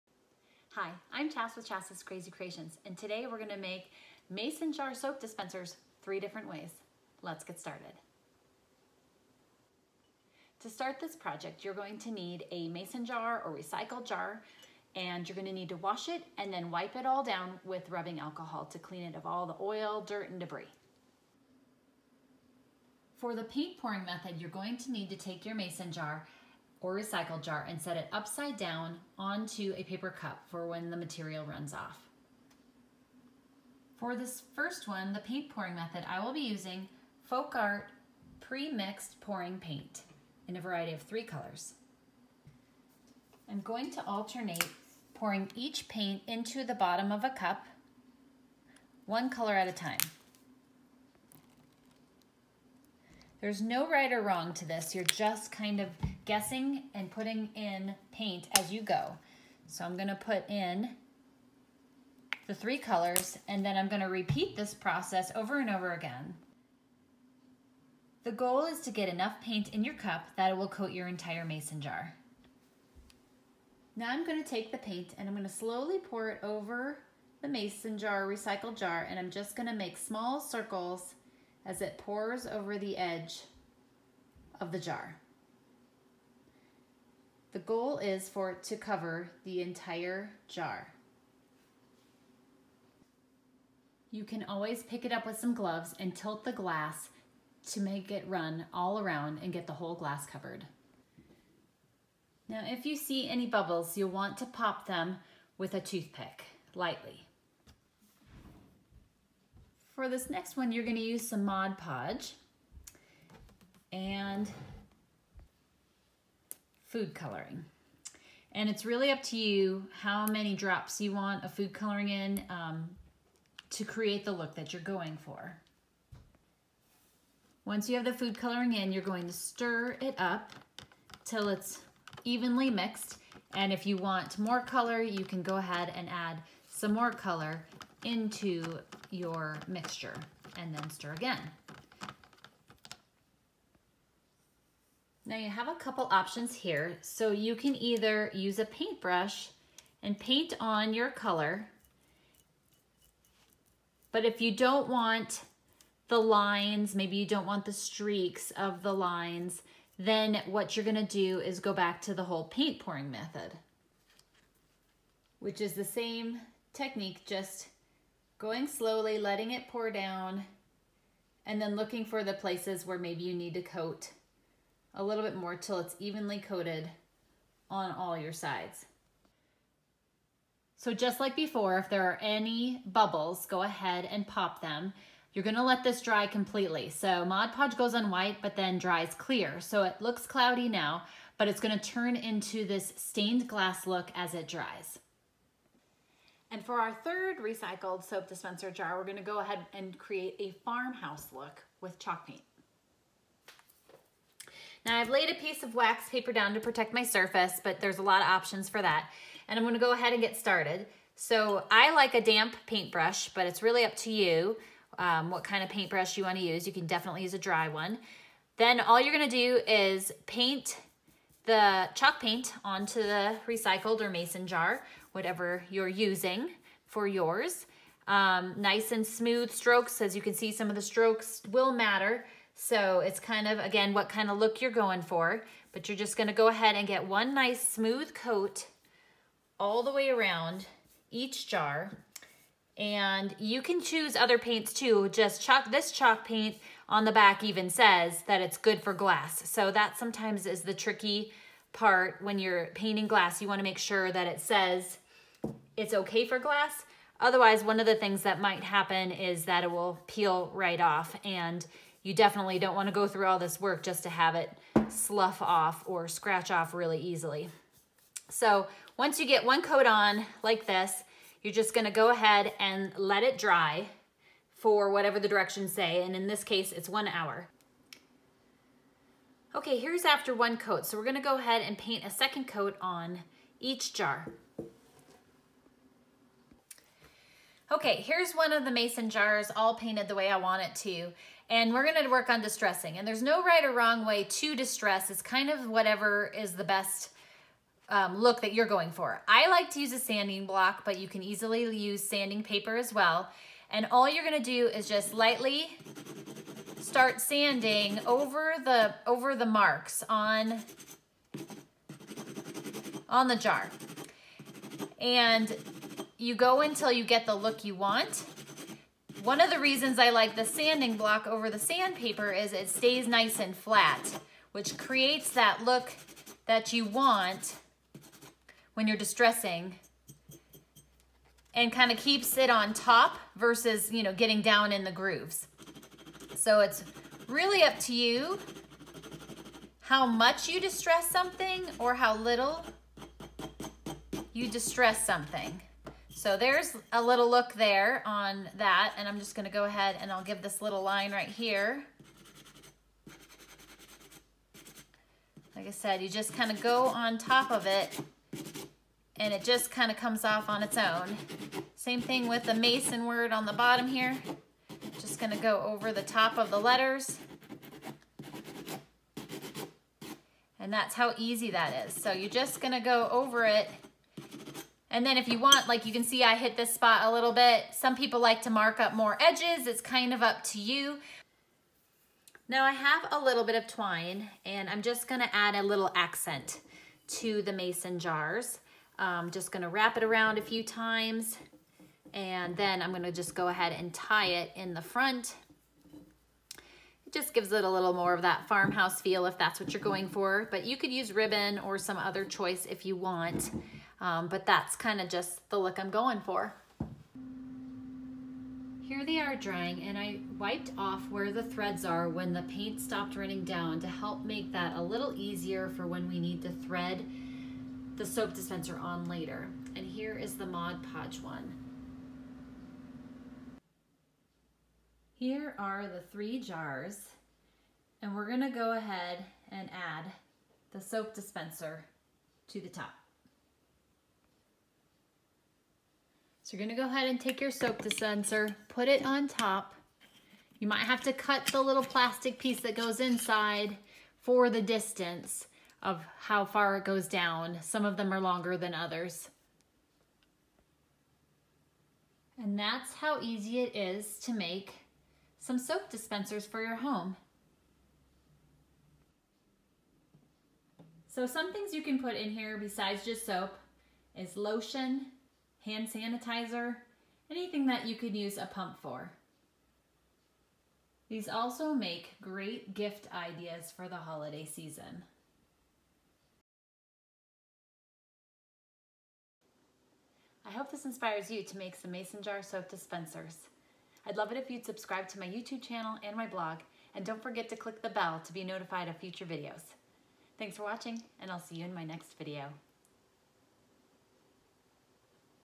DIY Hand Soap Dispenser
Did you know you can make your own hand soap dispenser?  I'm going to show you a few ways I made them using a mason or recycled jar!  This is a fantastic and useful DIY gift idea that everyone needs.

Materials
Soap Dispenser
Mason Jar
Chalk Paint
Mod Podge
Paint Pouring Medium